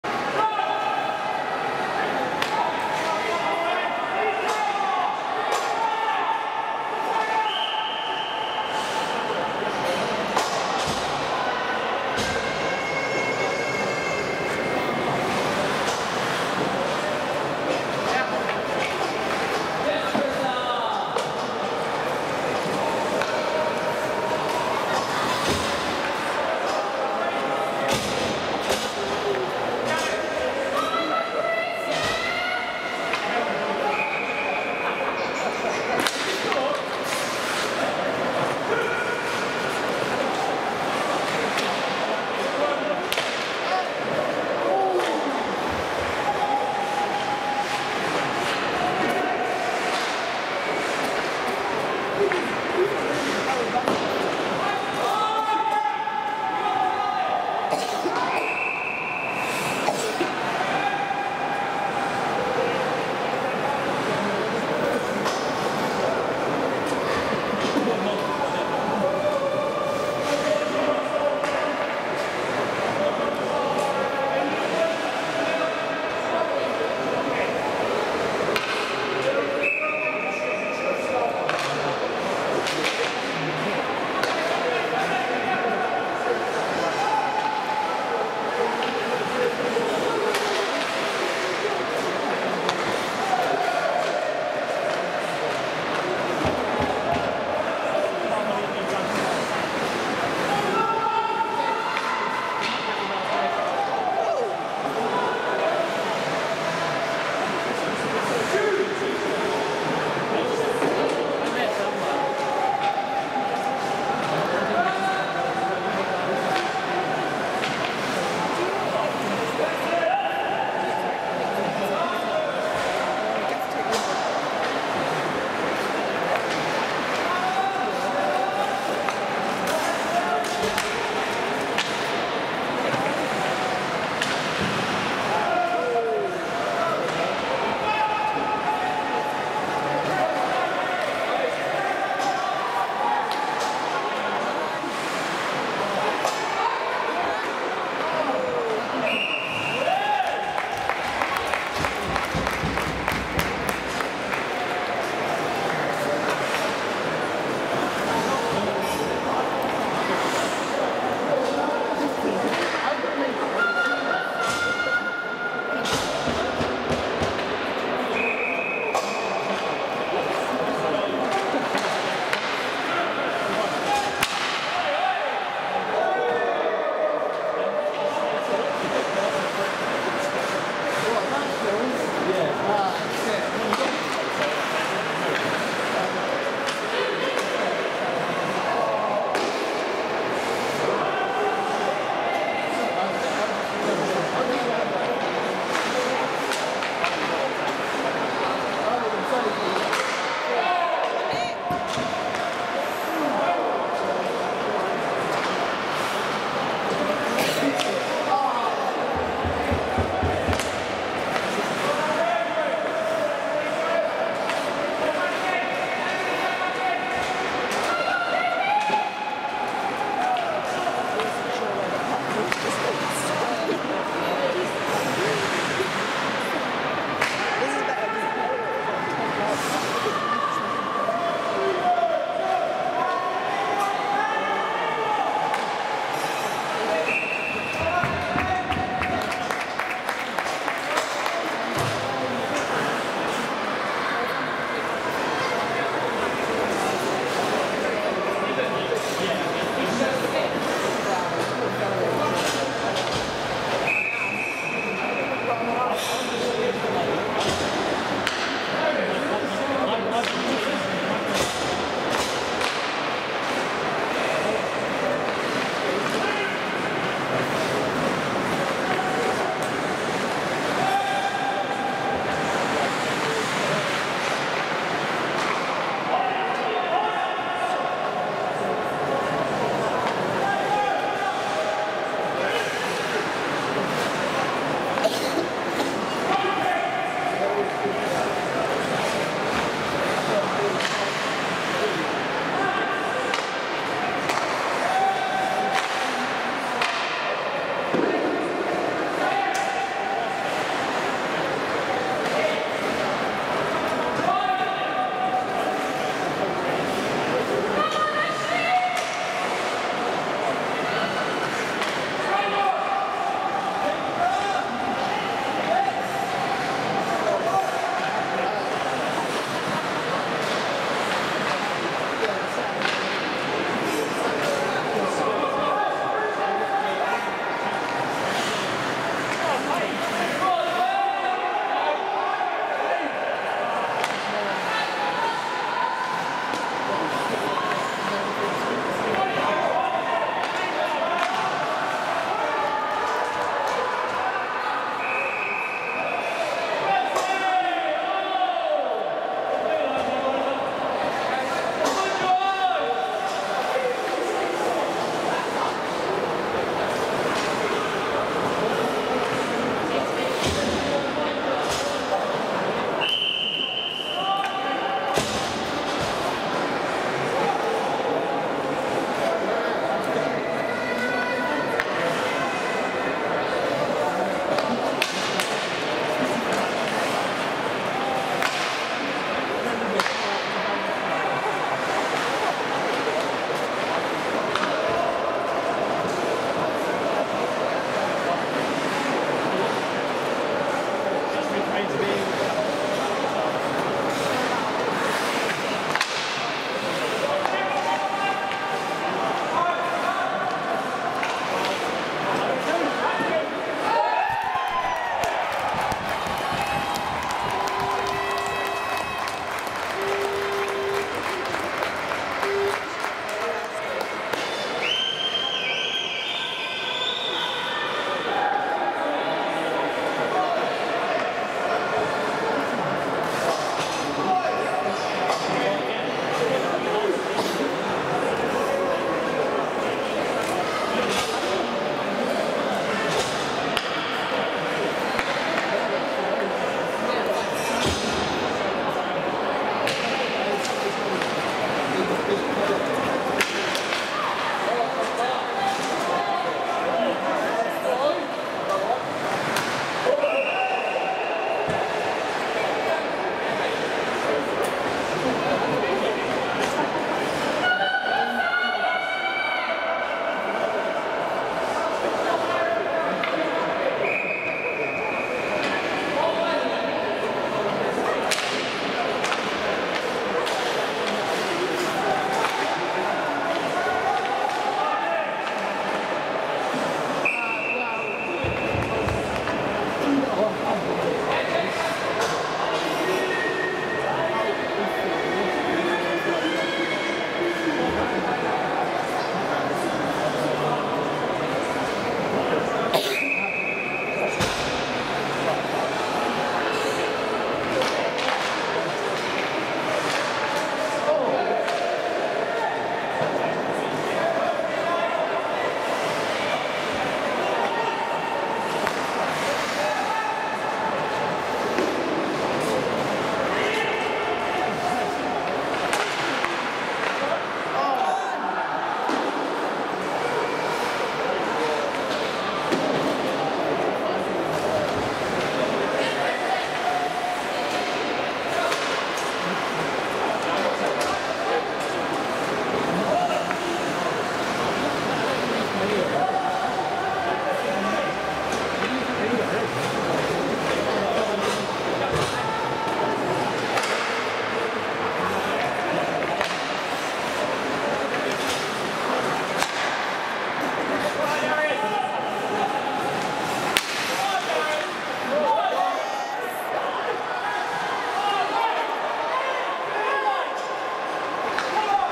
Storm Wreck Vs Nottingham Cyclones Period 2 Part 2
Storm Wreck took on the visiting Nottingham Cyclones. It is the first meeting of the 2 teams. Storm Wreck were looking to bounce back after defeat in their last game.